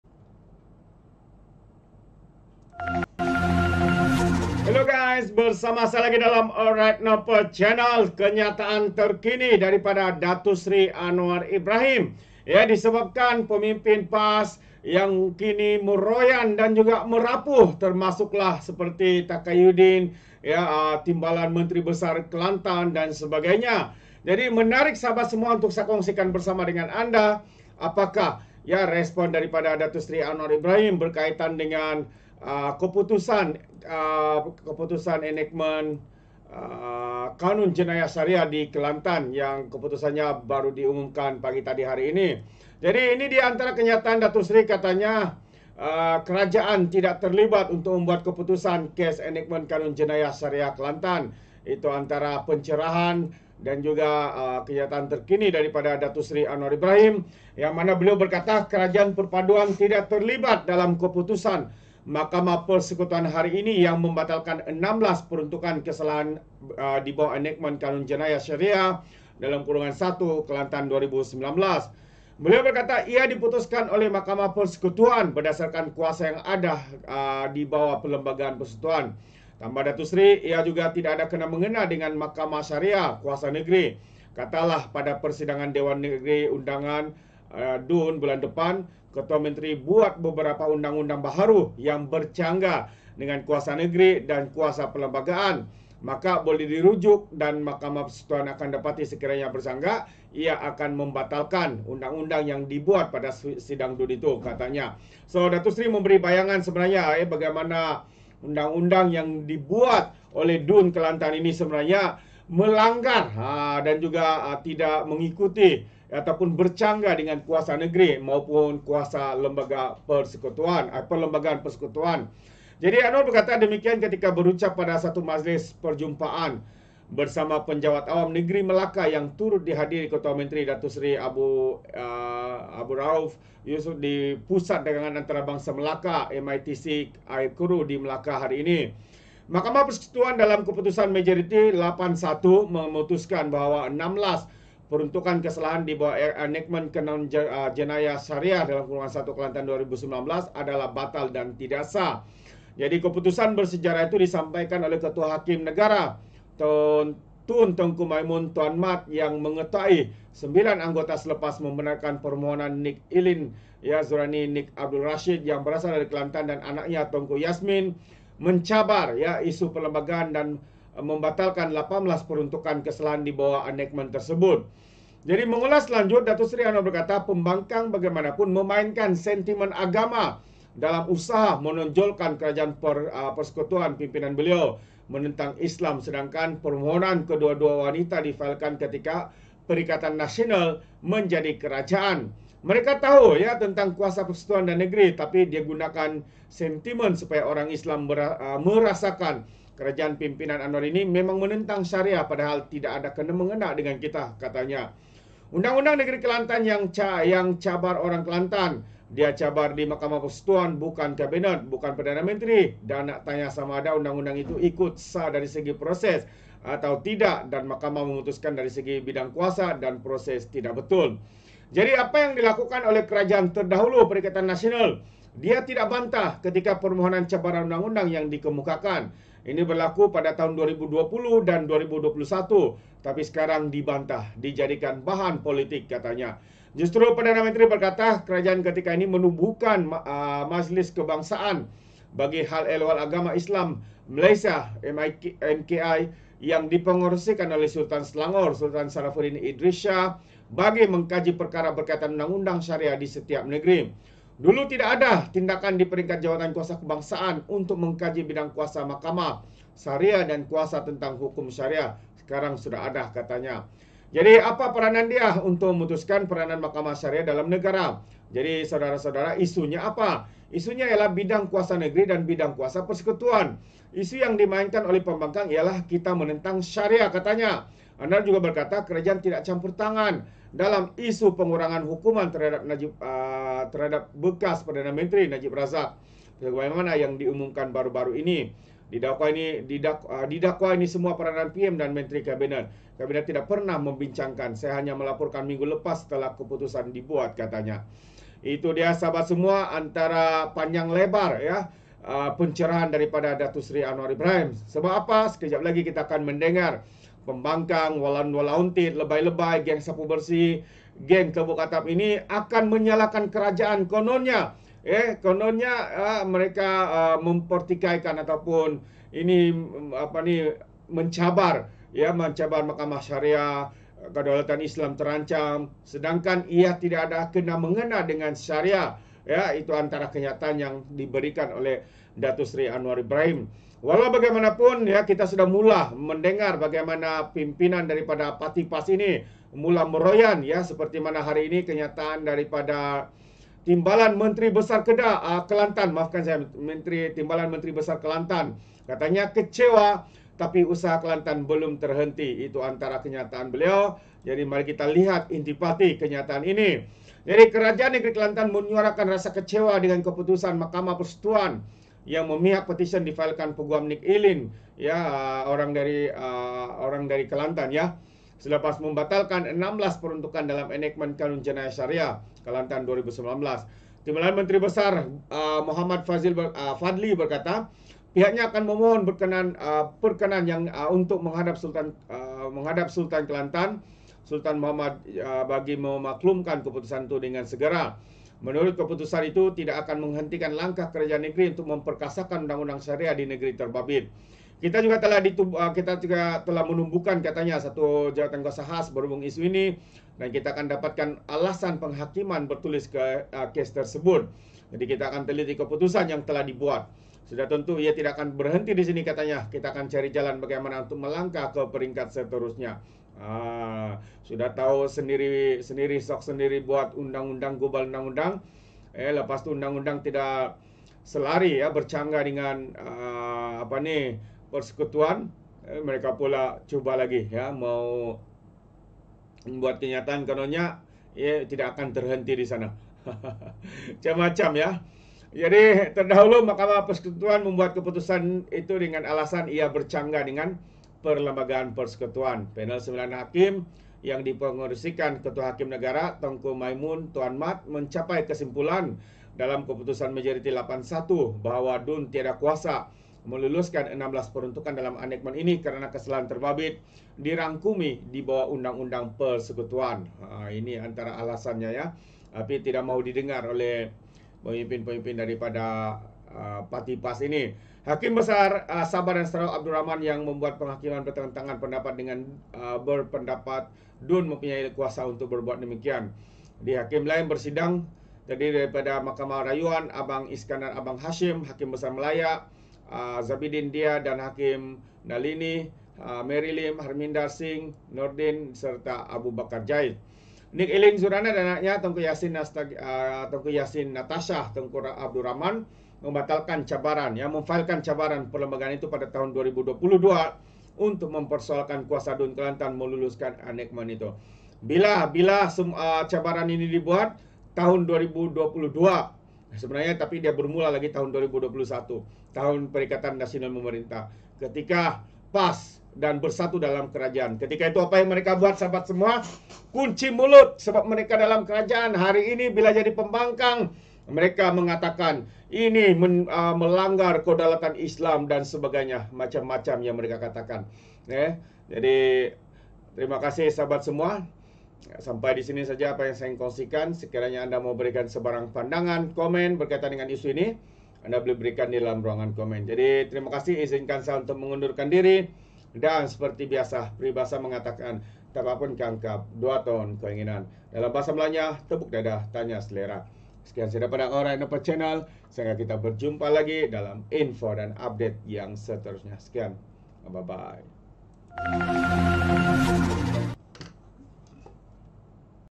KENYATAAN TERKINI DS ANWAR ISU PANAS DISEBABKAN PAS YANG MEROYAN & MERAPU!
Perdana Menteri Datuk Seri Anwar Ibrahim berkata Kerajaan Perpaduan tidak terlibat dalam keputusan Mahkamah Persekutuan hari ini yang membatalkan 16 peruntukan kesalahan di bawah Enakmen Kanun Jenayah Syariah (1) Kelantan 2019.
Beliau berkata ia diputuskan oleh Mahkamah Persekutuan berdasarkan kuasa yang ada di bawah Perlembagaan Persekutuan.
“Ia juga tidak ada kena-mengena dengan (Mahkamah) Syariah... kuasa negeri. Katalah (pada persidangan) Dewan (Undangan) Negeri (DUN) bulan depan, Ketua Menteri buat beberapa undang-undang baharu yang bercanggah dengan kuasa Negeri dan kuasa Perlembagaan Persekutuan maka boleh dirujuk dan Mahkamah Persekutuan akan dapati sekiranya bercanggah, akan membatalkan (undang-undang yang dibuat pada sidang DUN),” katanya.